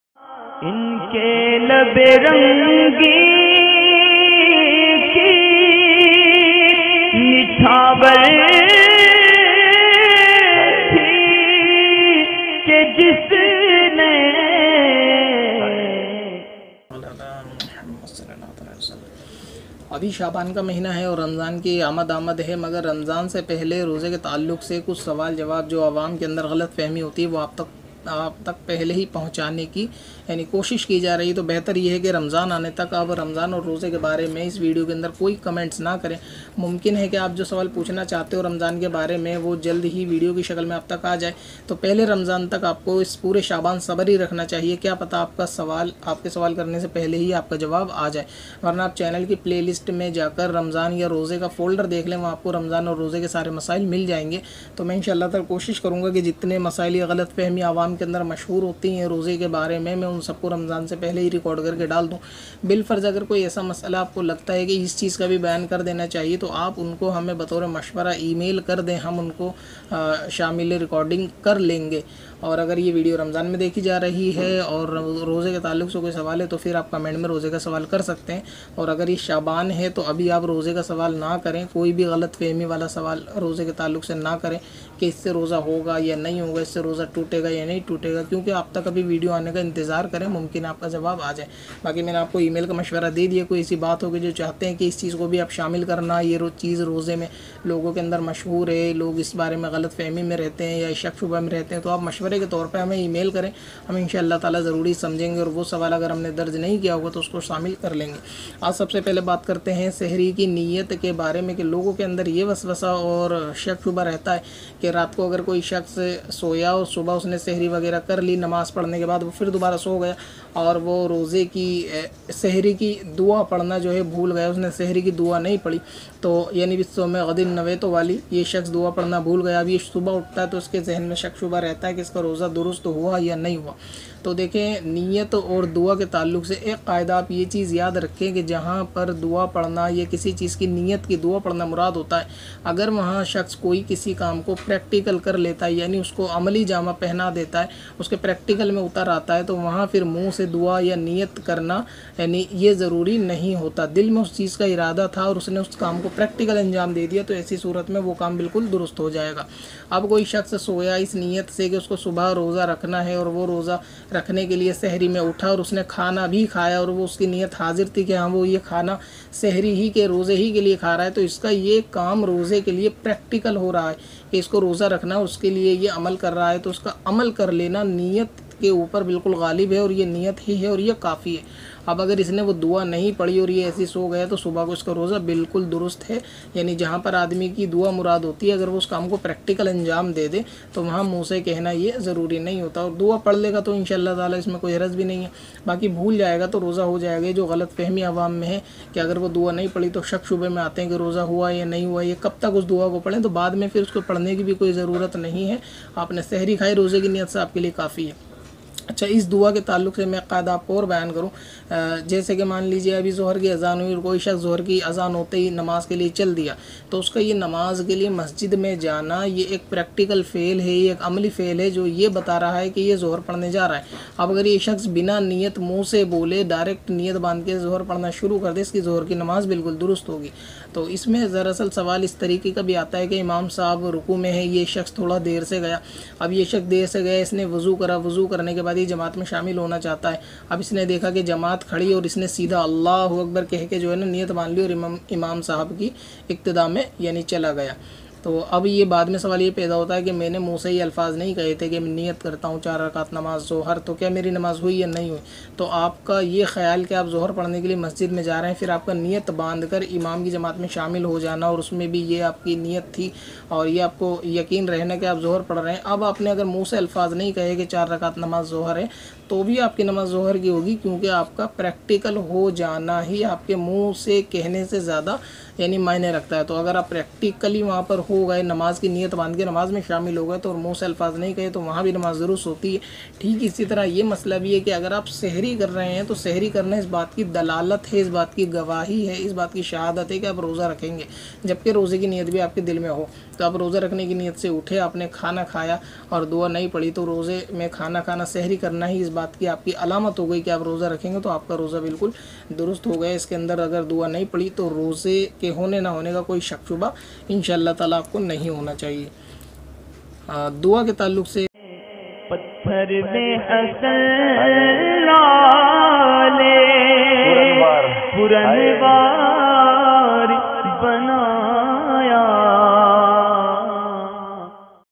Roze Ki Niyat Dua Sehri Me Bhool Jaye To Kya Roza Ho Jayega - Masail e Roza
(NEWS & Update) 📰 📅⏰
Aane Wali Videos & Live Timing Dekhen
roze ramazan taravih ke masail part 01 suwal 1 urdu sawal jawab

agar koi shakhs sehri me roze ki dua padhna bhoo jaye to roza ho jayega ya nahi,
अगर कोई सेहरी में रोज़े की दुआ पढ़ना भूल जाये या रोज़े की दुआ न पढ़े तो क्या रोज़ा हो जायेगा,